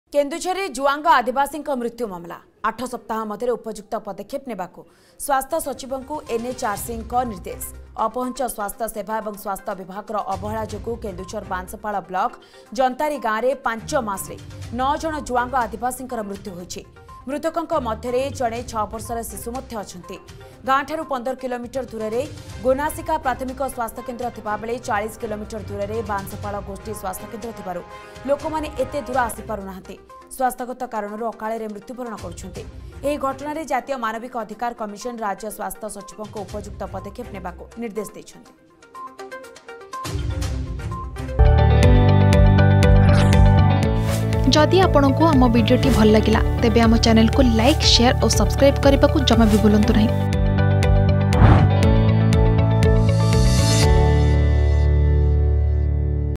୮ ସପ୍ତାହରେ ପଦକ୍ଷେପ ନିଅ : NHRC || Juanga Tribals Death Case || Keonjhar
କେନ୍ଦୁଝରରେ ଜୁଆଙ୍ଗ ଆଦିବାସୀଙ୍କ ମୃତ୍ୟୁ ମାମଲା । ୮ ସପ୍ତାହ ମଧ୍ୟରେ ଉପଯୁକ୍ତ ପଦକ୍ଷେପ ନେବାକୁ ସ୍ବାସ୍ଥ୍ୟ ସଚିବଙ୍କୁ ଏନଏଚଆରସିଙ୍କ ନିର୍ଦ୍ଦେଶ ।

୮ ସପ୍ତାହରେ ପଦକ୍ଷେପ ନିଅ : NHRC || Juanga Tribals Death Case || Keonjhar